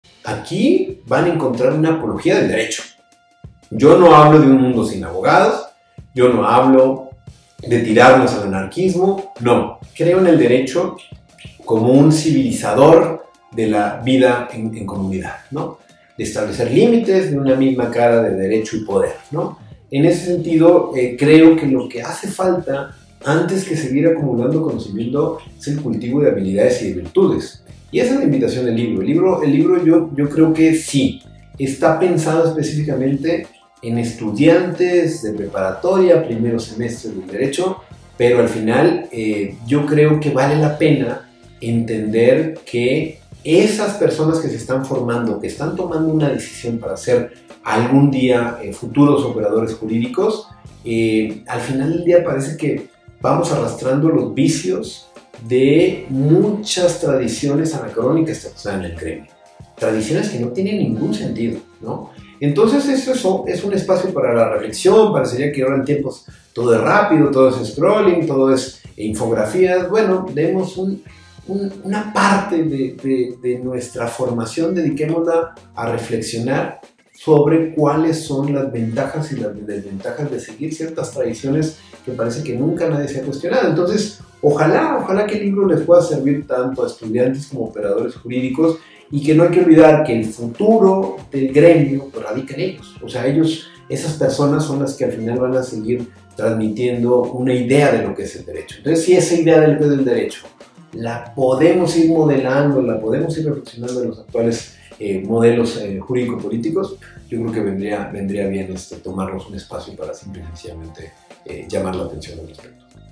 Una apología del Derecho | Tito Garza
La cantidad de instituciones educativas privadas que enseñan Derecho, va a la alza. Hay más abogados que perros en la calle, afirma Tito. En este contexto, en el que la licenciatura en Derecho es altamente demandada en las instituciones de educación superior en México, y en el que los conflictos parecen irresolubles por una falta de cultura de paz y, por ende, se requiere a la abogacía para coadyuvar para ese efecto, llama la atención que un investigador del Instituto de Investigaciones Jurídicas de la UNAM publique un título que parece un llamado a no estudiar la profesión.

Sin embargo, cuando uno se aproxima con cuidado al texto, se da cuenta que el exhorto es otro: estudiar el Derecho de una forma diferente. La pedagogía, la forma en la que se ejerce la abogacía y el objetivo con el que se hace, son algunos de los temas a los que Tito Garza se aproxima críticamente en su libro No estudies derecho (Pengüin Random House, 2023).

A propósito de esta reciente publicación, conversamos con Tito Garza, quien habló sobre la experiencia detrás de este libro, así como de las múltiples problemáticas que, con un lenguaje coloquial y valiéndose de muchas referencias de la cultura pop, permiten adentrarse a uno de tantos propósitos de la obra: la revisión de la función social de los abogados.